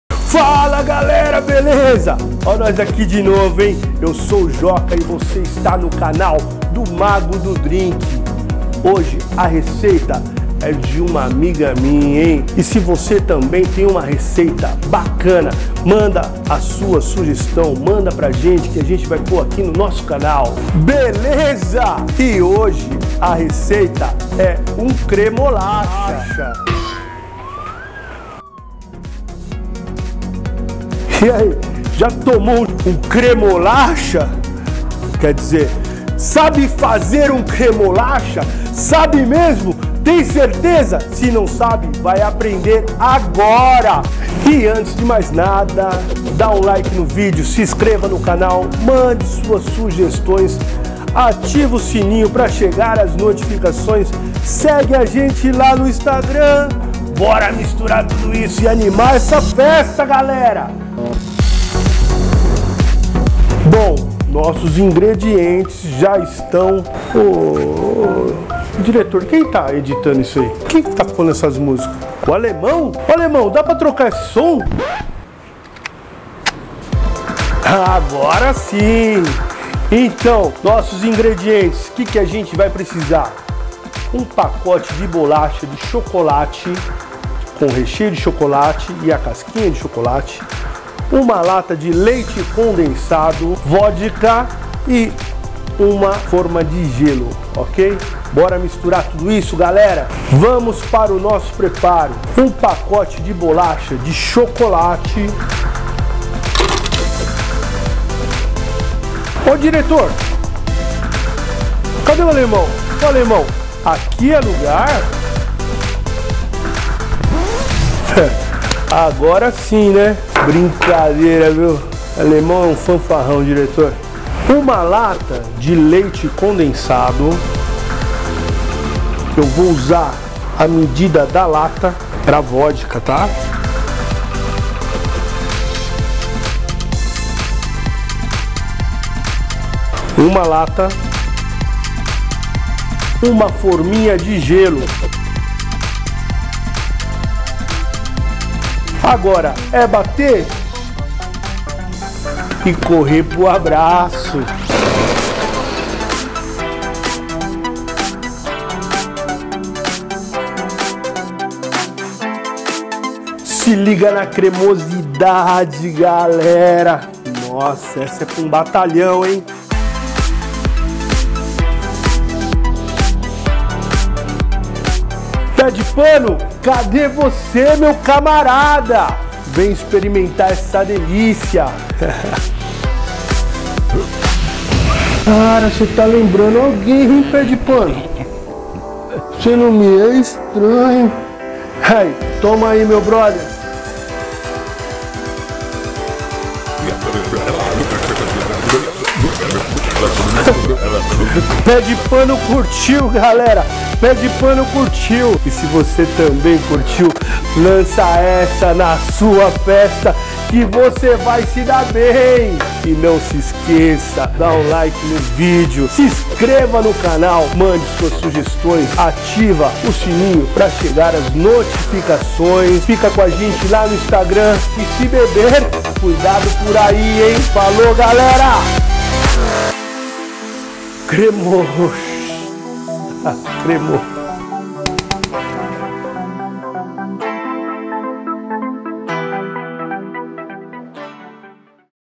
Mago do Drink - Cremolacha EP 4 T2 / Edição do Alemão
MAGO DO DRINK

Aprenda a fazer diversos drinks com as inúmeras receitas do “MAGO DO DRINK”.
Com diversão, você irá aprender a preparar drinks deliciosos de um jeito simples e sem frescuras.

“Lança essa na sua festa que você vai se dar bem” – Mago do Drink.

CREMOLACHA

Ingredientes:
1 pacote de bolacha de chocolate com recheio de chocolate.
Vodka
Leite condensado
Gelo

Esprema meio limão na coqueteleira, adicione 100 ml de groselha, 4 pedras de gelo, 1 colher rasa de açúcar e 1 ½ dose de cachaça. Agora é só misturas tudo isso e correr pro abraço galera! Divirta- se!